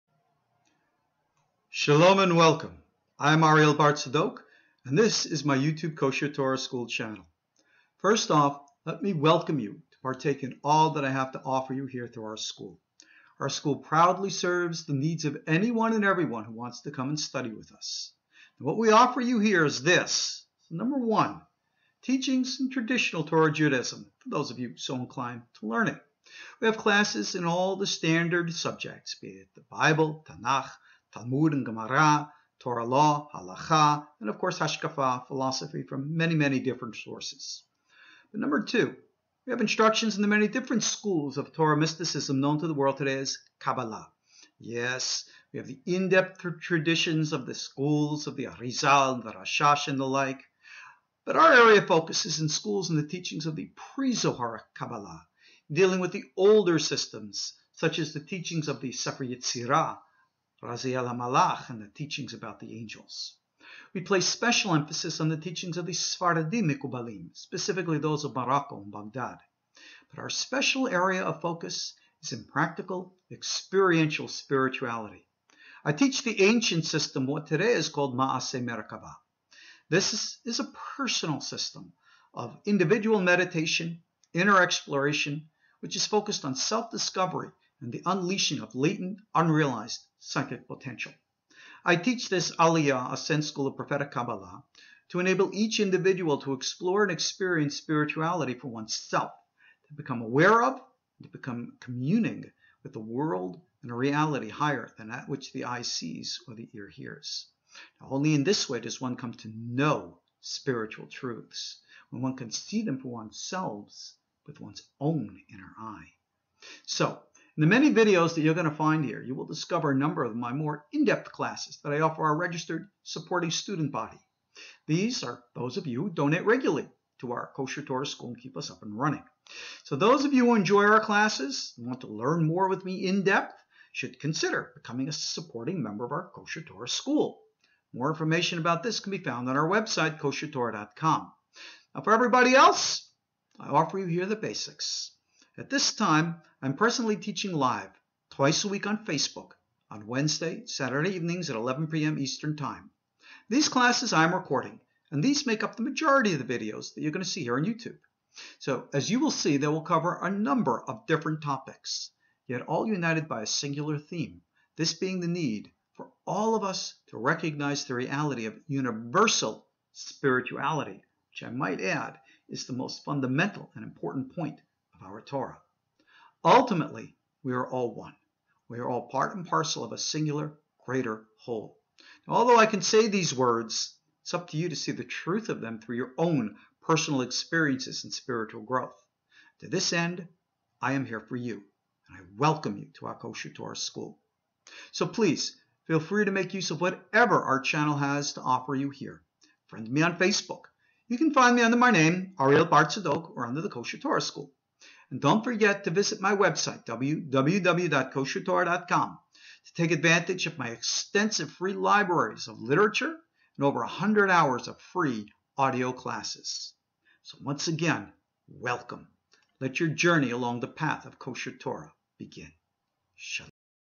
Shalom and Welcome to the KosherTorah School Channel.
Shalom, and welcome.
I am Ariel Bar Tzadok, and this is my You Tube KosherTorah School Channel.

First off, let me welcome you to partake in all that I have to offer here through our School.

Our School proudly serves the needs of anyone, and everyone who wants to come study with us.

What we offer you is this:

1. Teachings in traditional Torah Judaism, for those so inclined to learn it.

We have classes in all the standard subjects,
be it the Bible, the Talmud, Torah Law, and Hashkafa/philosophy.

2. Instruction in the many different schools of Torah mysticism, known to the world as Kabbalah.

Yes, we have in-depth teachings on the traditional Schools of the Ari'zal, but our area of focus is in the school and teachings of the pre-Zoharic Kabbalah, dealing with the older systems, such as the Sefer Yetzirah, and the teachings about the Angels.

We place special emphasis on the teachings of the Sephardic Kabbalists, specifically those of Morocco, and of Baghdad.

3. Our special area of focus is in practical, experiential spirituality.  I teach the ancient system of what today is called Ma'aseh Merkava.

This is a system of meditation, and inner exploration which is focused on self-discovery, and the unleashing of latent, unrealized psychic potential.

I teach this Aliyah/ascent school of Prophetic Kabbalah to enable each individual to explore and experience spirituality for oneself, to become aware of, and to commune with a world and reality higher than that which the eye sees, or the ear hears.

Only in this way does one come to know spiritual truths, when one can see them for oneself with one's own inner eye.

So, in the many videos that you will find here, you will discover a number of my more in-depth classes that I offer to our registered, supporting student body.

These are those of you who donate regularly to our KosherTorah School to keep us up and running.

Those of you who enjoy our classes and want to learn with me more in-depth should consider becoming a supporting member of our KosherTorah School.

For everyone else, I offer here the basics.  At this time, I am presently teaching live, twice a week on Facebook, on Wednesday and Saturday evenings at 11 p.m. Eastern time.

These classes I am recording, and these make up the majority of videos that you will see here on YouTube.

As you will see they will cover a number of different topics, yet all united by a singular theme, this being the need for us all to recognize the reality of universal spirituality.

Ultimately, we are all one!  We are all part and parcel of a singular greater whole.

 Although I can say these words, it is up to you to see the truth of them through your own personal experiences, and spiritual growth.

 To this end, I am here for you, and I welcome you to our KosherTorah School.

So, please feel free to make use of whatever our Channel has to offer you here.

Friend me on Facebook.  You can find me under my name Ariel Bar Tzadok or under the KosherTorah School.

to take advantage of my extensive free libraries of literature, and over 100 hours of free audio classes.

Once again welcome!  Let your journey along the path of KosherTorah begin.